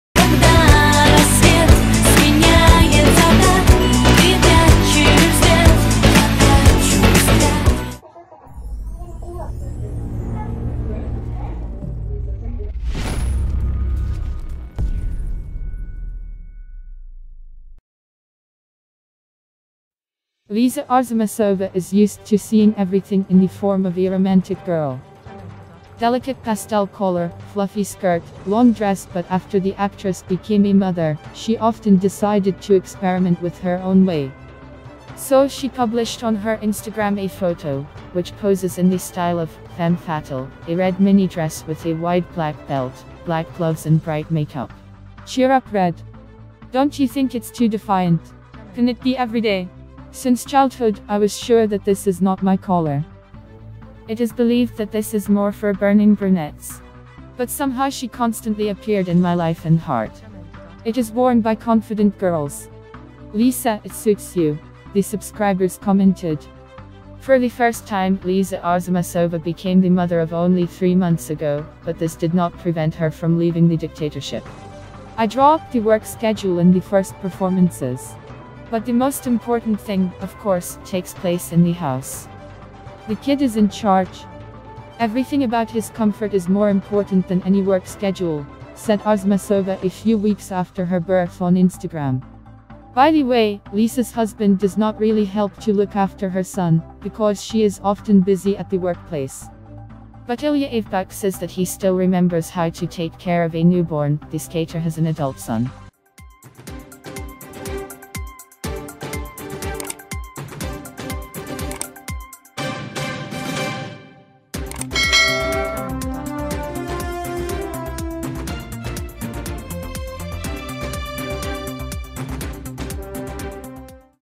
Too sexy outfit | Liza Arzamasova decided on a bold experiment
Liza Arzamasova is used to seeing everything in the form of a romantic girl.
Delicate pastel color, fluffy skirt, long dress
But after the actress became a mother, she often decided to experiment with her own way.

So, she published on her Instagram a photo, which poses in the style of "femme fatale": a red mini-dress with a wide black belt, black gloves and bright makeup.
“Cheer up red ... Don't you think it's too defiant? Can it be everyday?

Since childhood, I was sure that this is not my color. It is believed that this is more for burning brunettes.
But somehow she constantly appeared in my life and heart.

For the first time, Liza Arzamasova became the mother of only 3 months ago, but this did not prevent her from leaving the dictatorship.
“I draw up the work schedule and the first performances.
But the most important thing, of course, takes place in the house. The kid is in charge!
Everything about his comfort is more important than any work schedule, - said Arzamasova a few weeks after her birth on Instagram.
By the way, Lisa's husband does not really help to look after her son, because she is often busy at the workplace.
But Ilya Avebukh says that he still remembers how to take care of a newborn, the skater has an adult son.